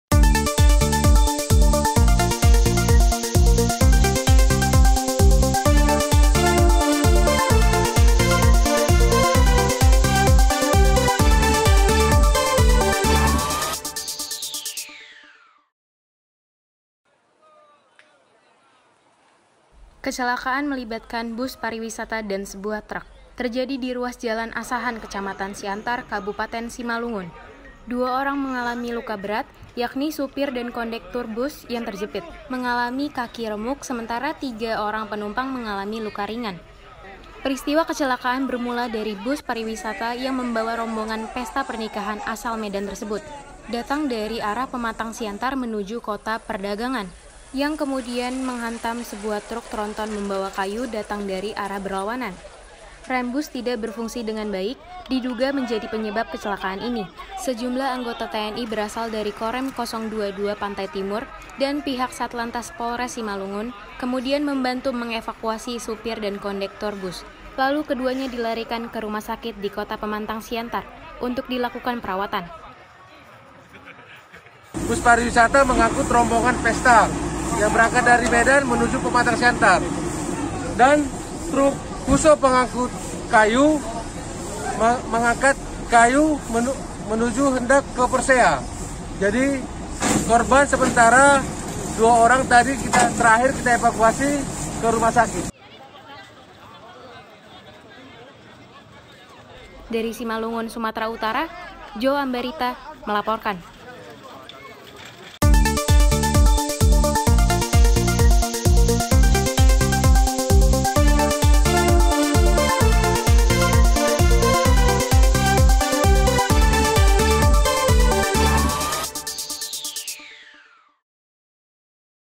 REM BLONG BUS ROMBONGAN PESTA SERUDUK TRUK KAYU
Kecelakaan melibatkan bus pariwisata dan truk terjadi di ruas Jalan Asahan, Kecamatan Siantar, Kabupaten Simalungun. 2 orang mengalami luka berat yakni sopir dan kondektur bus yang terjepit mengalami kaki remuk, sementara 3 orang penumpang mengalami luka ringan.

Peristiwa kecelakaan bermula dari bus pariwisata yang membawa rombongan pesta pernikahan asal Medan tersebut datang dari arah Pematangsiantar menuju Kota Perdagangan yang kemudian menghantam sebuah truk tronton membawa kayu datang dari arah berlawanan.

Rem bus tidak berfungsi dengan baik, diduga menjadi penyebab kecelakaan ini. Sejumlah anggota TNI yang berasal dari Korem 022 Pantai Timur dan pihak Satlantas Polres Simalungun kemudian membantu mengevakuasi sopir dan kondektur bus yang terjepit lalu keduanya dilarikan ke rumah sakit di Kota Pematangsiantar untuk dilakukan perawatan.

Tim Liputan : Joe Ambarita

Melaporkan.